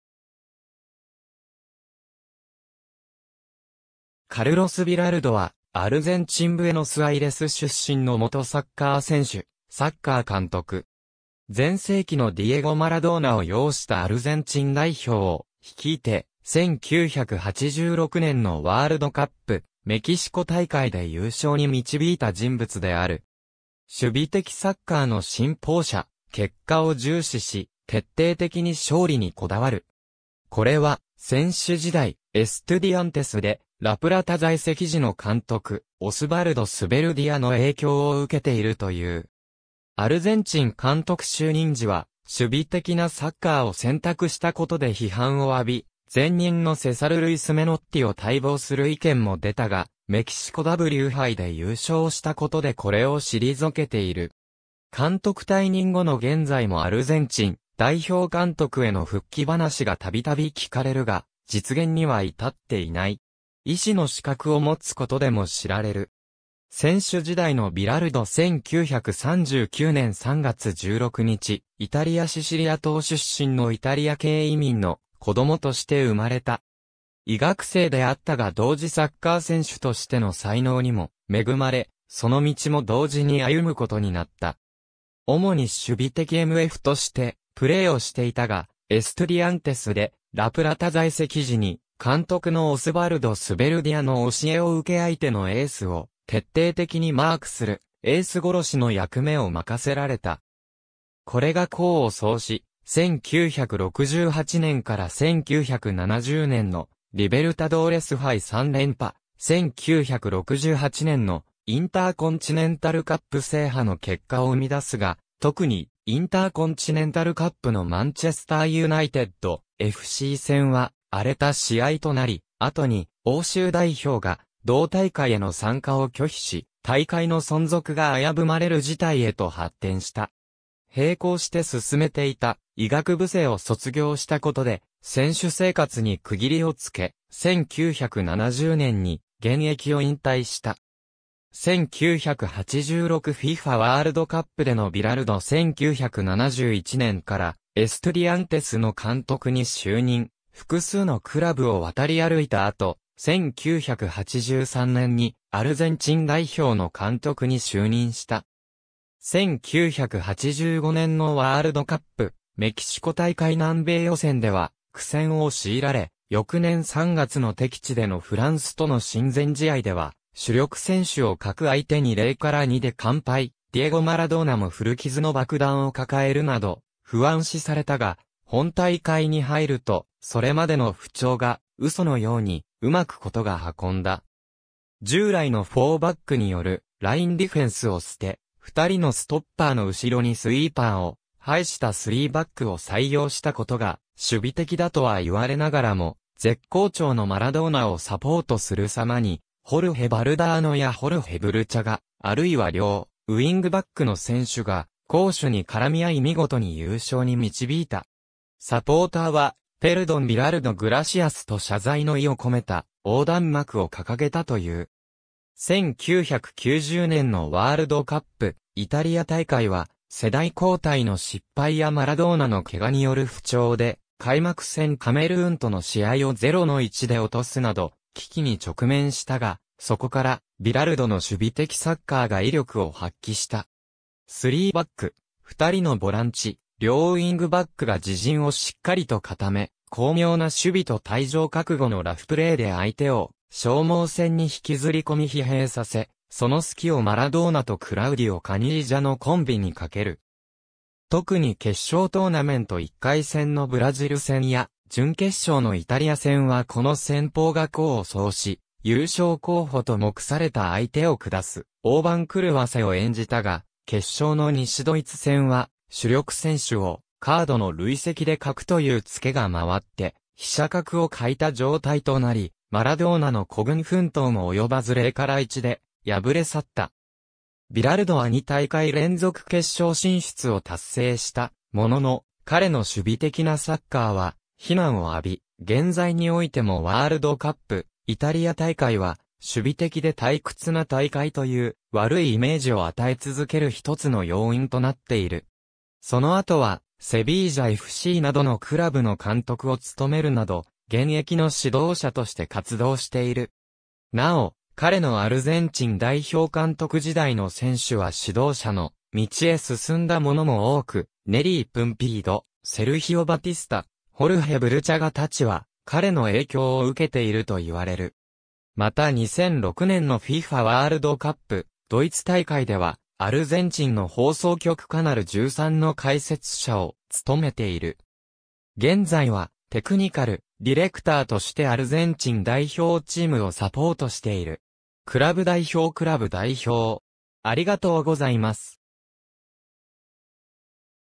カルロス・ビラルド
CAサン・ロレンソ・デ・アルマグロの選手
エストゥディアンテス・デ・ラ・プラタの選手
デポルティーボ・カリの監督
カルロス・ビラルド（Carlos Salvador Bilardo, 1939年3月16日 - ）は、アルゼンチン・ブエノスアイレス出身の元サッカー選手、サッカー監督。
全盛期のディエゴ・マラドーナを擁したアルゼンチン代表を率いて、1986年のワールドカップ・メキシコ大会で優勝に導いた人物である。
守備的サッカーの信奉者、結果を重視し徹底的に勝利に拘る。
これは選手時代、エストゥディアンテス・デ・ラ・プラタ在籍時の監督オスバルド・スベルディアの影響を受けているという。
アルゼンチン監督就任時は守備的なサッカーを選択した事で批判を浴び、前任のセサル・ルイス・メノッティを待望する意見も出たがメキシコW杯で優勝した事でこれを退けている。
監督退任後の現在もアルゼンチン代表監督への復帰話が度々聞かれるが実現には至っていない。
医師の資格を持つことでも知られる。
選手時代のビラルド 1939年3月16日イタリア・シシリア島出身のイタリア系移民の子供として生まれた。
医学生であったが同時サッカー選手としての才能にも恵まれ、その道も同時に歩む事になった。
主に守備的MFとしてプレーをしていたが、エストゥディアンテス・デ・ラ・プラタ在籍時に、監督のオスバルド・スベルディアの教えを受け相手のエースを徹底的にマークする「エース殺し」の役目を任せられた。
これが功を奏し、1968年から1970年のリベルタドーレス杯三連覇、1968年のインターコンチネンタルカップ制覇の結果を生み出すが、特にインターコンチネンタルカップのマンチェスター・ユナイテッドFC戦は荒れた試合となり、後に欧州代表が同大会への参加を拒否し、大会の存続が危ぶまれる事態へと発展した。
並行して進めていた医学部生を卒業した事で選手生活に区切りを付け、1970年に現役を引退した。
1986 FIFAワールドカップでのビラルド 1971年からエストゥディアンテスの監督に就任、複数のクラブを渡り歩いた後、1983年にアルゼンチン代表の監督に就任した。
1985年のワールドカップ・メキシコ大会南米予選では苦戦を強いられ、翌年3月の敵地でのフランスとの親善試合では、主力選手を欠く相手に0-2で完敗、ディエゴ・マラドーナも古傷の爆弾を抱えるなど不安視されたが、本大会に入るとそれまでの不調が嘘の様に上手く事が運んだ。
従来の4バックによるラインディフェンスを棄て、2人のストッパーの後ろにスイーパーを配した3バックを採用した事が守備的だとは言われながらも、絶好調のマラドーナをサポートする様にホルヘ・バルダーノやホルヘ・ブルチャガ、或いは両ウイングバックの選手が攻守に絡み合い見事に優勝に導いた。
サポーターは「Perdon Bilardo Gracias」と謝罪の意を込めた横断幕を掲げたという。
1990年のワールドカップ・イタリア大会は、世代交代の失敗やマラドーナの怪我による不調（それでもパサーとしては相手に充分過ぎる程の脅威を与えた）で、開幕戦カメルーンとの試合を0-1で落とすなど危機に直面したが、そこからビラルドの守備的サッカーが威力を発揮した。
3バック、2人のボランチ、両ウイングバックが自陣をしっかりと固め、巧妙な守備と退場覚悟のラフプレーで相手を消耗戦に引きずり込み疲弊させ、その隙をマラドーナとクラウディオ・カニージャのコンビに賭ける。
特に決勝トーナメント1回戦のブラジル戦や準決勝のイタリア戦はこの戦法が功を奏し、優勝候補と目された相手を下す大番狂わせを演じたが、決勝の西ドイツ戦は主力選手をカードの累積で欠くという附けが回って飛車角を欠いた状態となり、マラドーナの孤軍奮闘も及ばず0-1で敗れ去った。
ビラルドは2大会連続決勝進出を達成したものの、彼の守備的なサッカーは非難を浴び、現在においてもワールドカップ・イタリア大会は守備的で退屈な大会という悪いイメージを与え続ける一つの要因となっている。
その後は、セビージャFCなどのクラブの監督を務めるなど現役の指導者として活動している。
なお、彼のアルゼンチン代表監督時代の選手は指導者の道へ進んだ者も多く、ネリー...